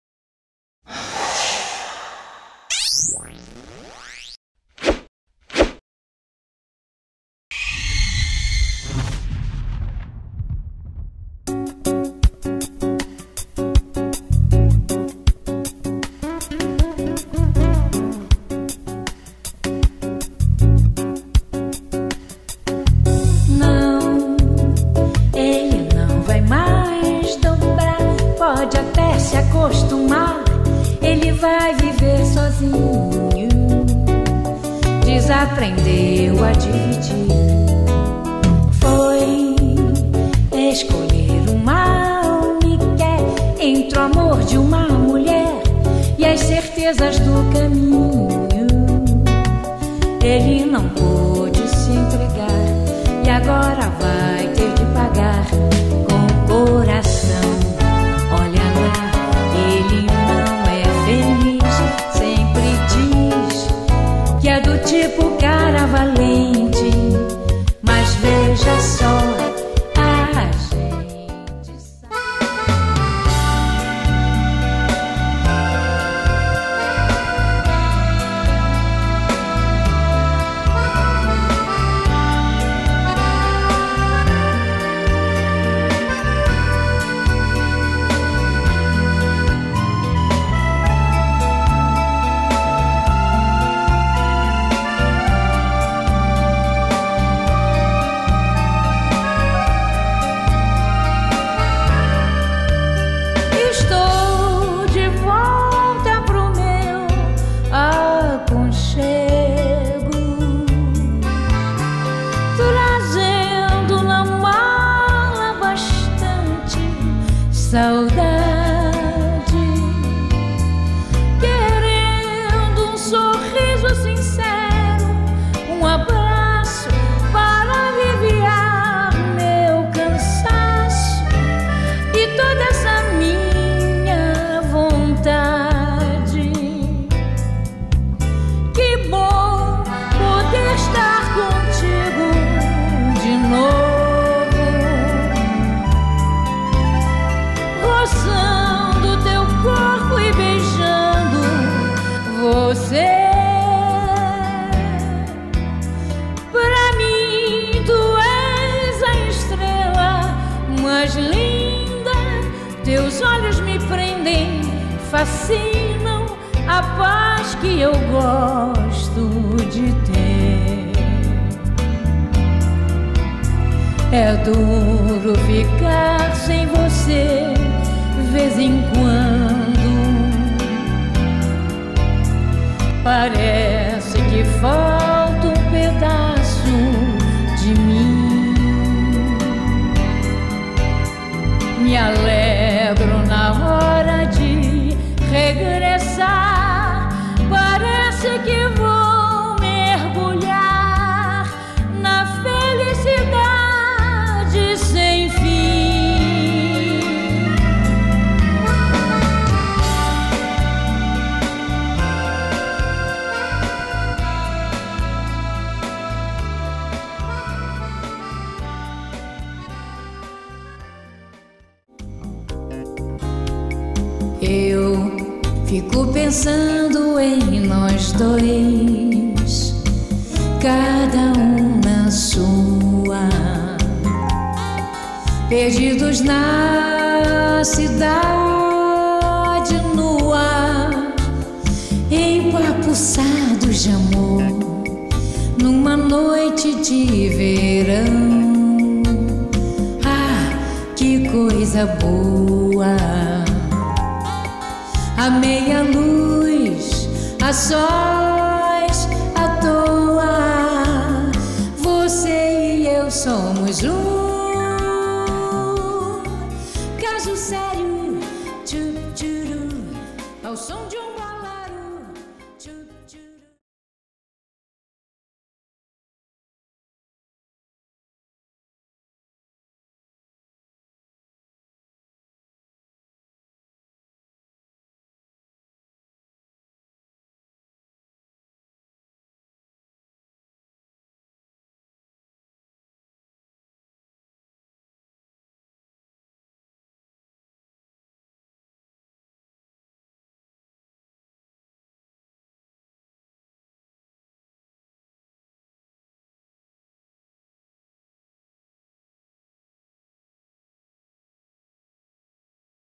CANTORA MYRINHA MORAES E SUA BANDA BM COPA NO CLUBE MILITAR - RJ
A CANTORA MYRINHA MORAES SE APRESENTA  JUNTAMENTE COM SUA BANDA BM COPA, NO CLUBE MILITAR NO BAILE DAS QUINTAS. DAS 18:00hs ÁS 22:00hs. END - AV RIO BRANCO 251 - CENTRO - RIO DE JANEIRO - RJ - BRASIL. INF - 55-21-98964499